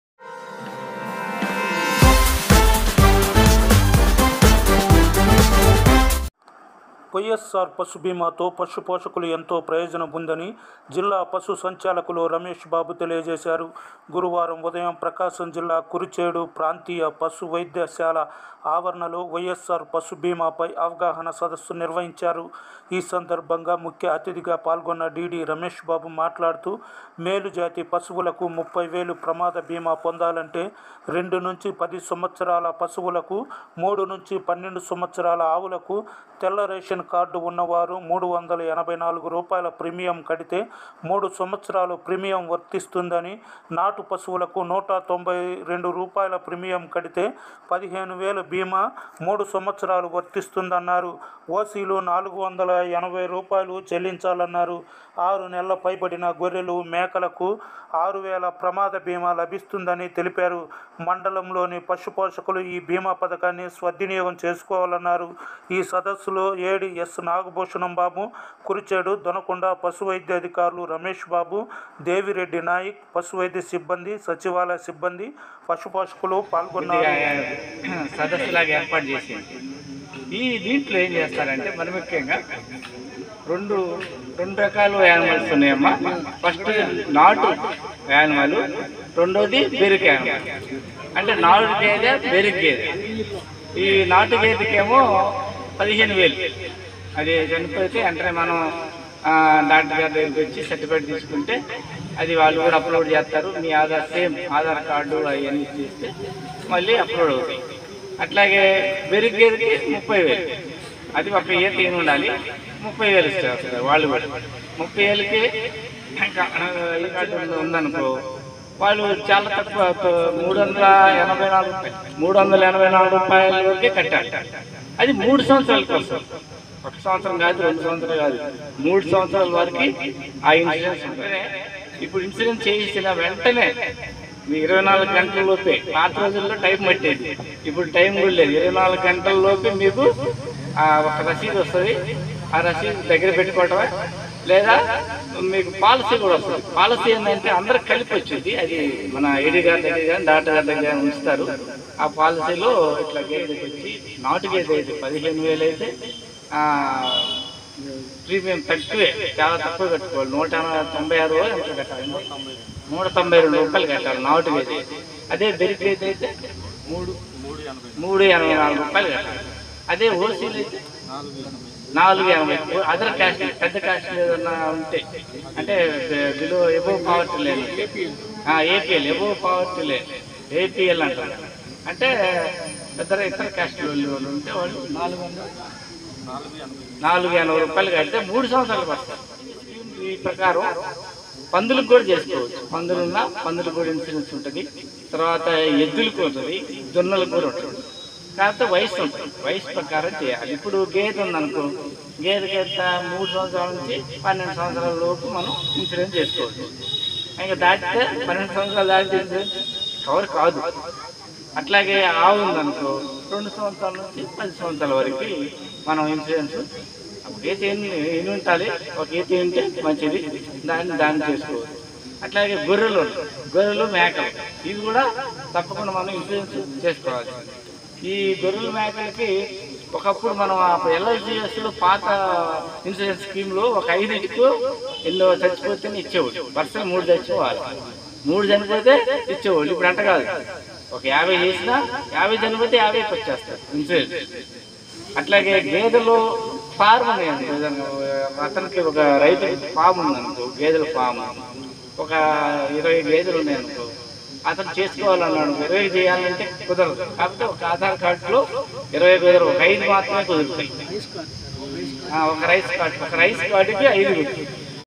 వైయస్సార్ పశు భీమాతో పశు పోషకులకు ఎంతో మేలు -DD రమేష్ బాబు|AP NEWS 90
AP NEWS 90#ap news 90#APNEWS90#apnews90#ap news90Telugu#ap news 90 Live#@AP NEWS 90
వైయస్సార్ పశు భీమాతో పశు పోషకులకు ఎంతో మేలు చేకూరుతుందని జిల్లా పశు సంచాలకులు రమేష్ బాబు తెలిపారు కురిచేడు పశువైద్య శాల ఆవరణలో గురువారం ఉదయం వైయస్సార్ పశు భీమా పై అవగాహన సదస్సు నిర్వహించారు

Santhosh Kumar kola
D-N-5-135 besid Sri Raghavendra high school